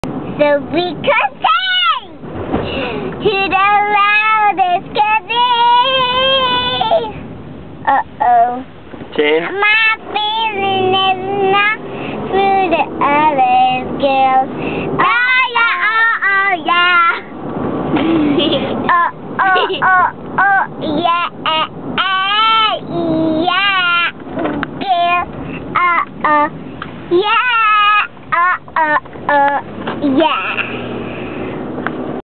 Rylie Singing in the Car
Rylie loves to sing - she's so silly!